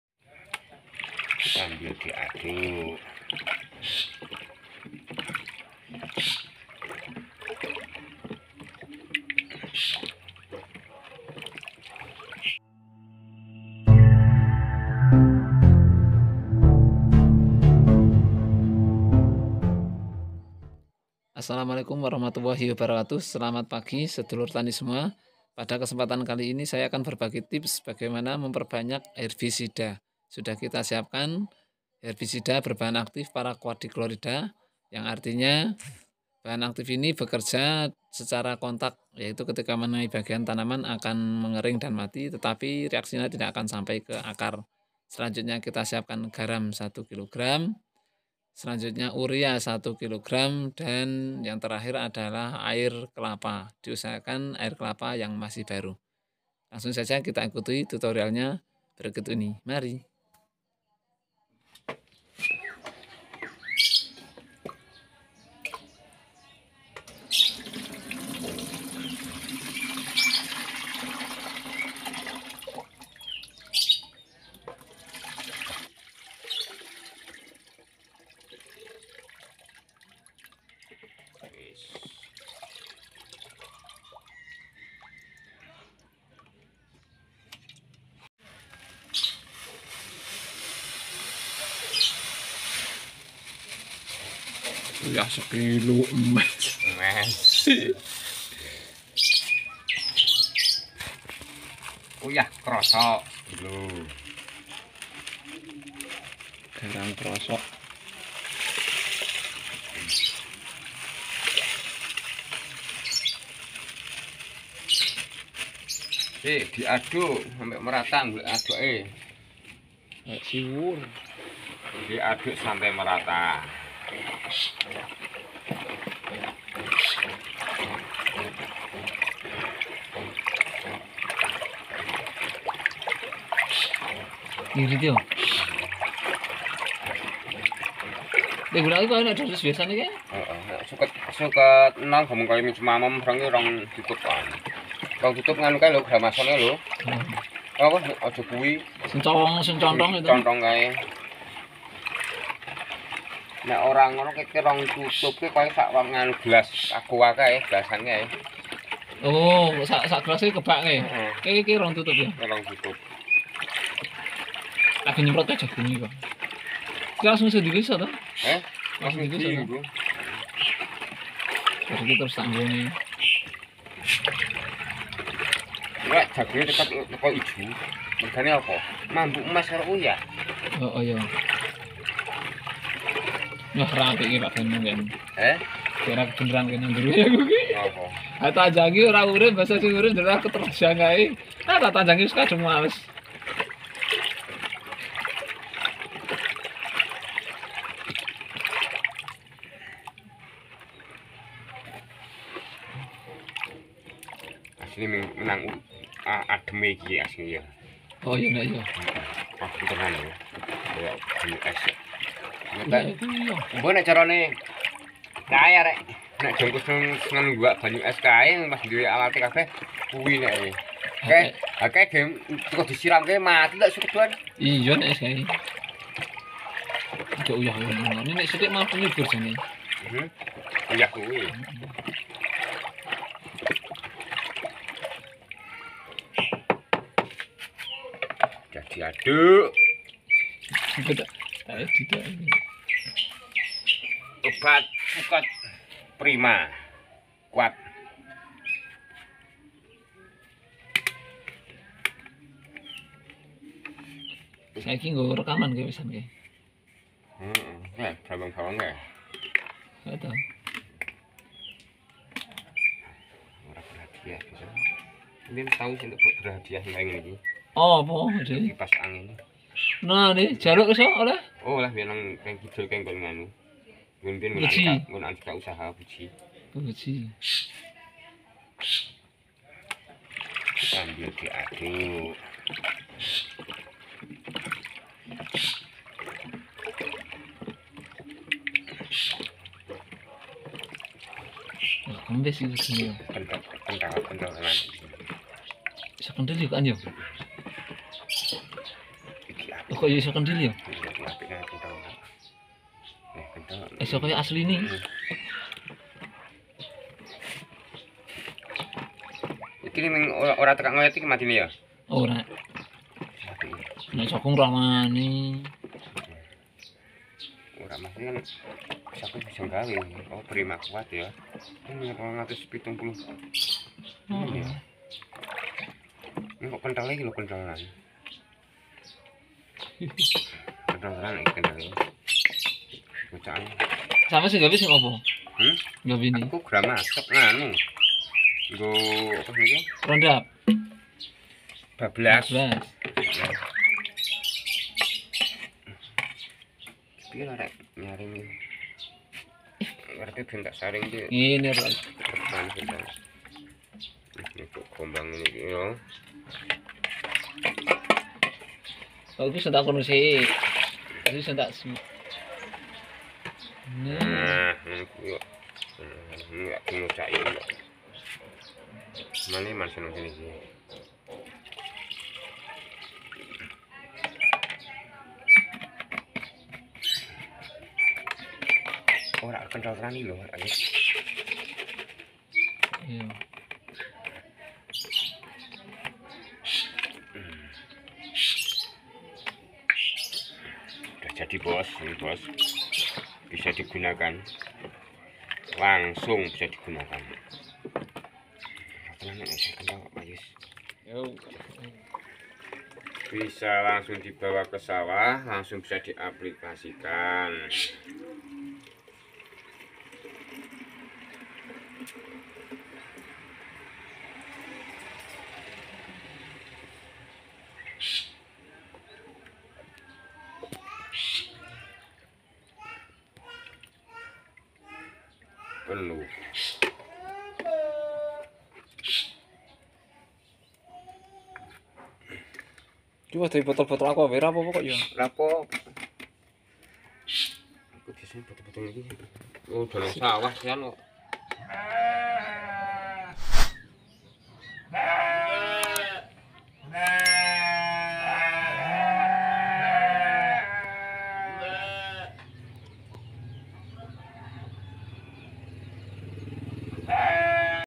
CARA MEMPERBANYAK HERBISIDA MURAH DAN MUDAH
Cara memperbanyak herbisida murah dan murah adalah solusi para petani di masa seperti sekarang ini yang harga herbisida sangat tinggi. ini adalah salah satu solusi bagi para petani untuk mengatasi nya. bahan yang di buruhkan untuk membuat herbisida yang murah dan mudah ini yaitu Herbisida 1 liter, garam krosok 1 kilo gram, urea 1 kilogram, dan air kelapa 5 liter.
pertama tama air kelapa kita saring dulu kemudian dimasukkan urea 1 kilo gram dan di aduk. Setelah tercampur, dimasukan garam 1 kg dan aduk lagi. setelah di aduk merata baru di masukkan herbisida. Setelah tercampur, herbisida sudah bisa di gunakan dengan dosis 200ml per untuk 16 liter air. Dengan racikan ini 1 liter herbisida bisa menjadi 7 liter. sangat murah dan mudah. semoga bermanfaat..